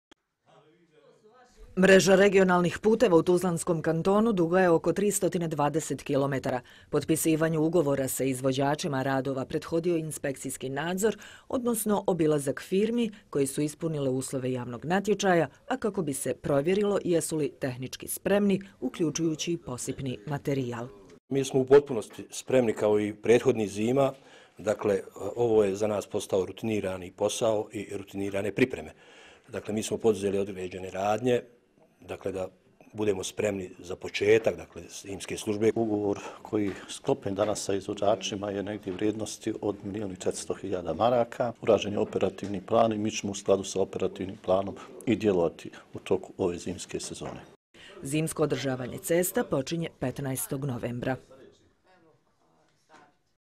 Ugovori s firmama koje će obavljati zimsko održavanje cesta u TK
TUZLA, 7. novembra (FENA) - Ugovori s firmama koje će obavljati zimsko održavanje cesta na regionalnim putevima u TK potpisani su u Direkciji regionalnih cesta.